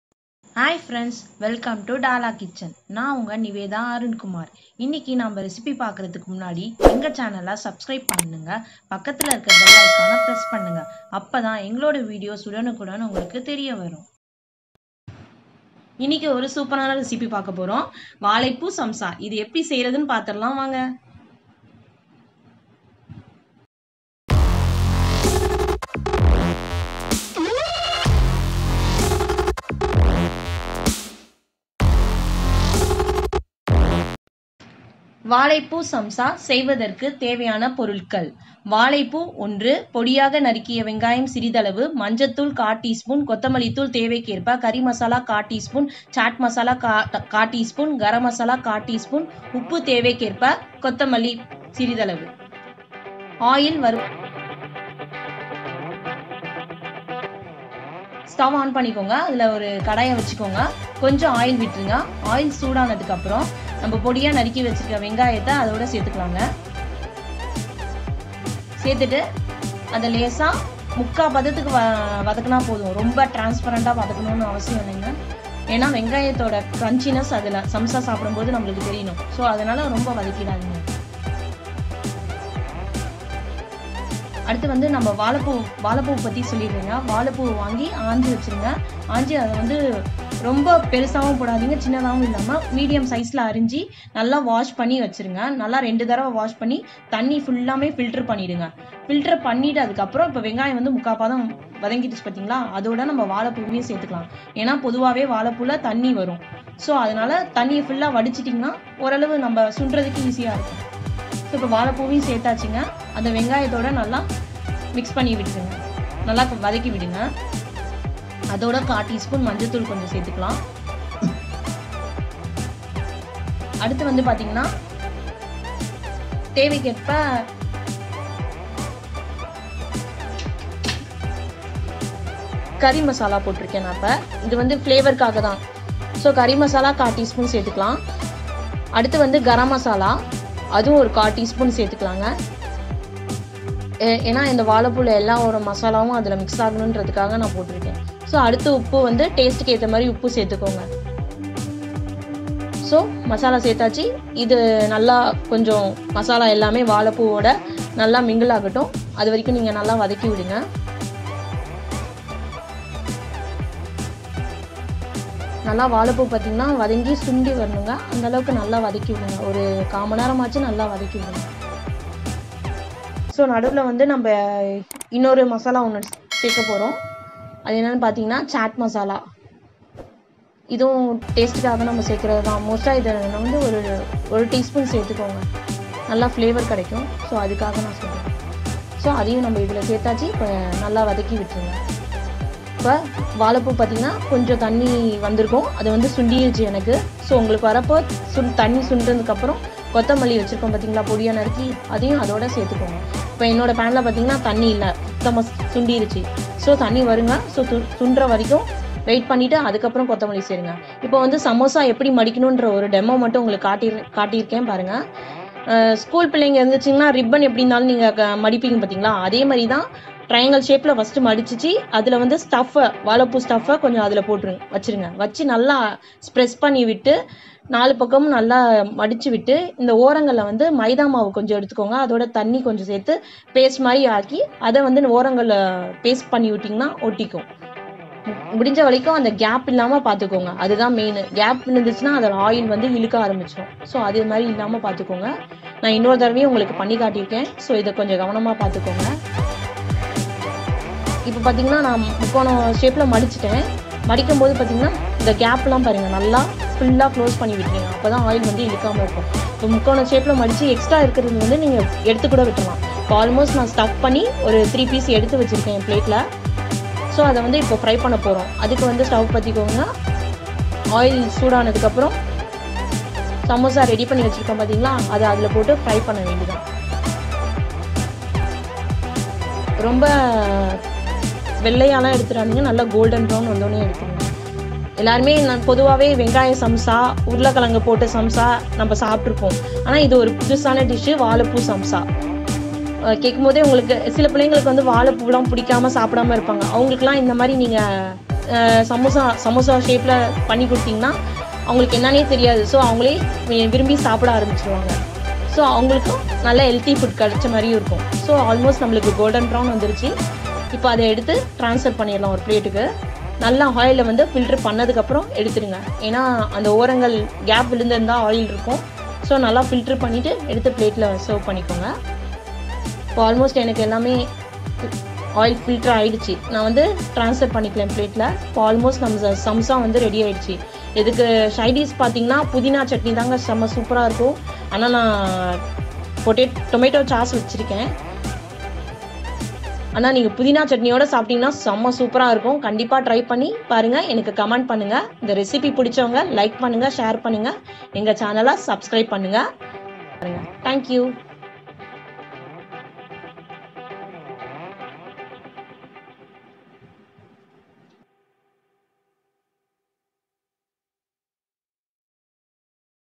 banana flower samosa ||வாழைப்பூ சமோசா|| vaalaipoo samosa recipe in tamil
In this video we will see how to make Vaalaipoo samosa in tamil.
வாழைப்பூ சமோசா  செய்வதற்கு  தேவையான பொருட்கள்
                                  வாழைப்பூ 1
   பொடியாக நறுக்கிய வெங்காயம்  சிறிதளவு
                    மஞ்சள் தூள் 1/4   டீஸ்பூன்
                கொத்தமல்லி  தேவைக்கு ஏற்ப
                  கரி மசாலா தூள் 1 / 4  டீஸ்பூன்
                     சாட் மசாலா தூள்  1 / 4  டீஸ்பூன்
                      கரம் மசாலா தூள்  1 / 4  டீஸ்பூன்
                         உப்பு   தேவைக்கு ஏற்ப
                                        ஆயில்
Also please do share your feedback about the recipe in the comments below. All the best and happy and enjoying cooking!